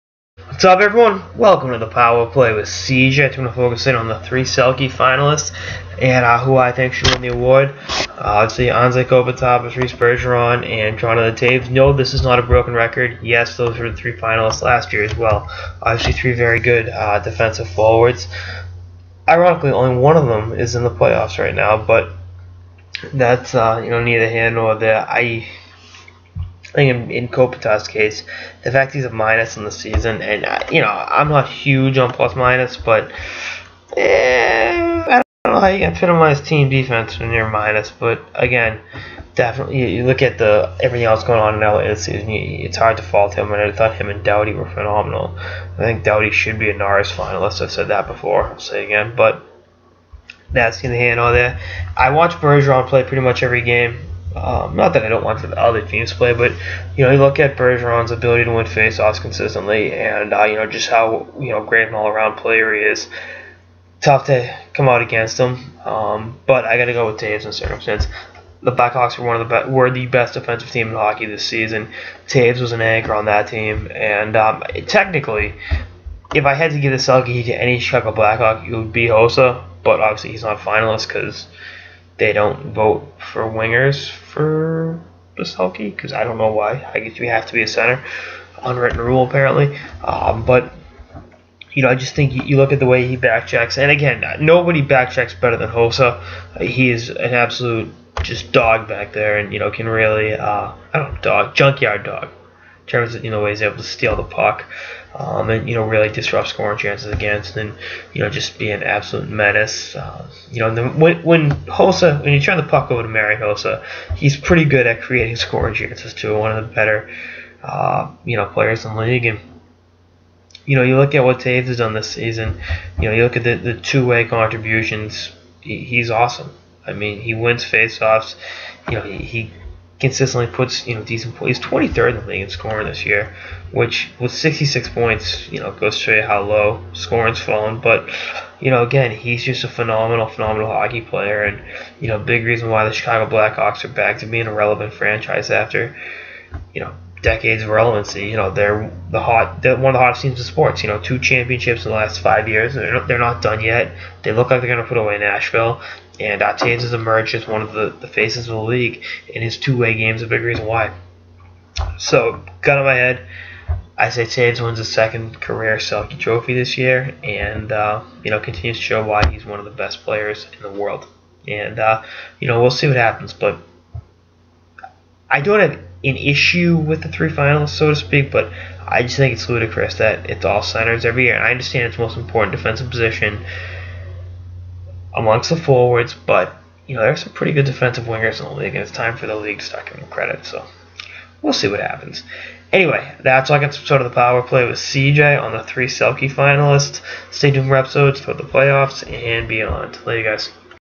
Power Play with CJ: Selke Finalists
CJ dishes on the three finalists and why he has a certain Chicago Blackhawk taking it home